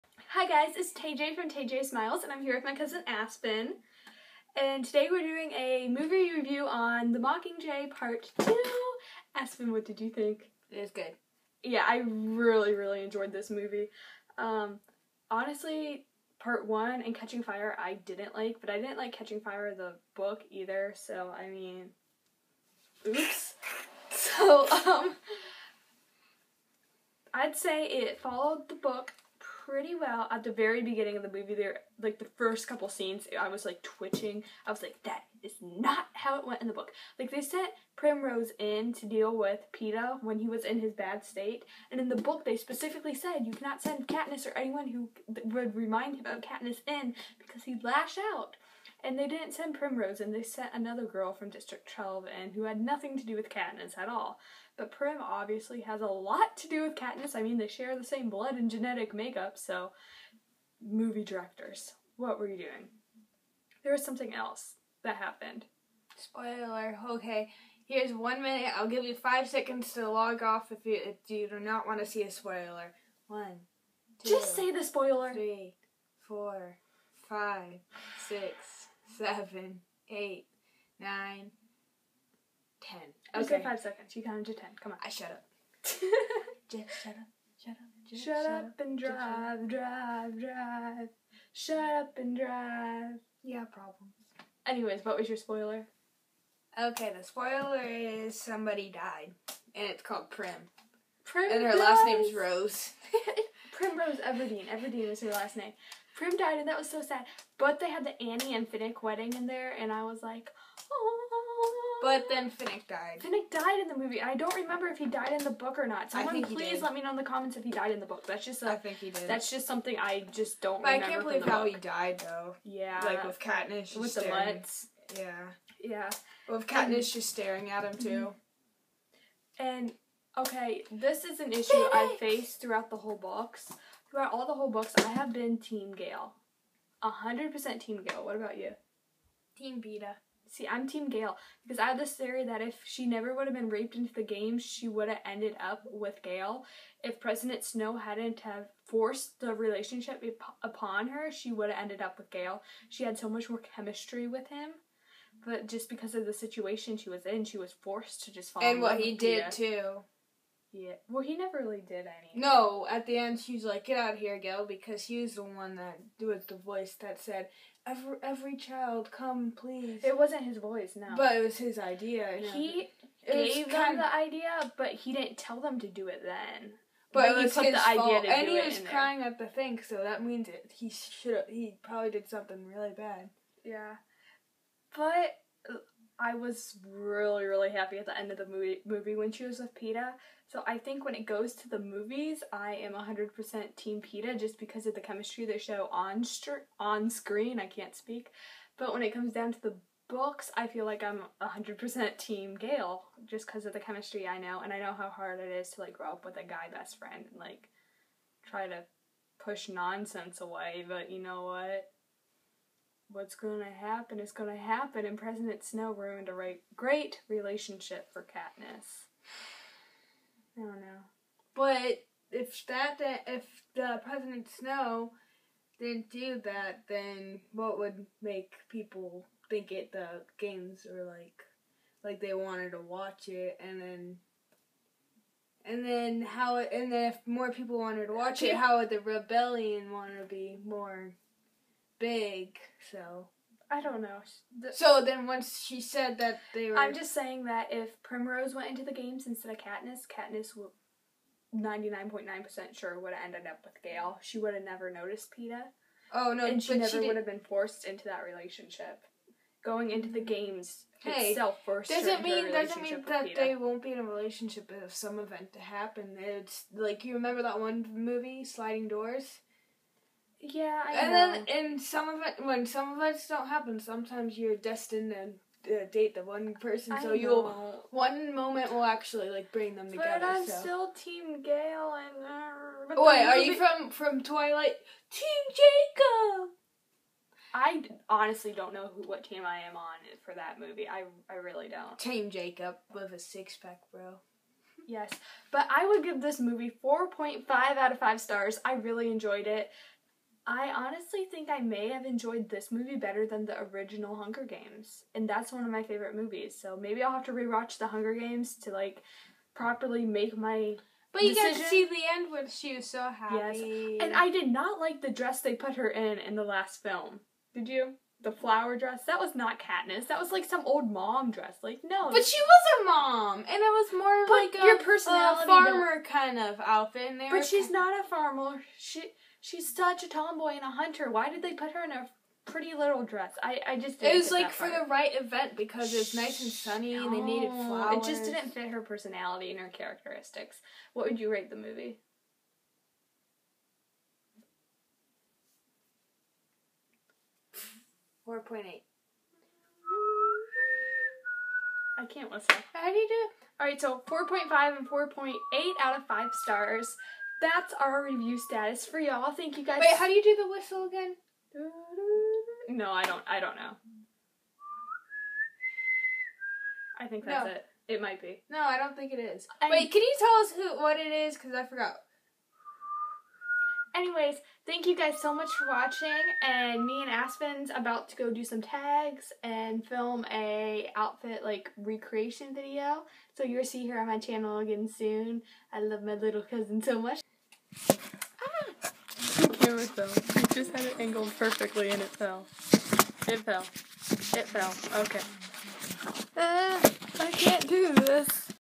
Mockingjay part 2! Movie review!
A Few Spoilers, watch at your own risk.
I enjoyed the movie alot, what about you?
Video #2 with My Cousin Aspen! DOnt you love her, I know I do.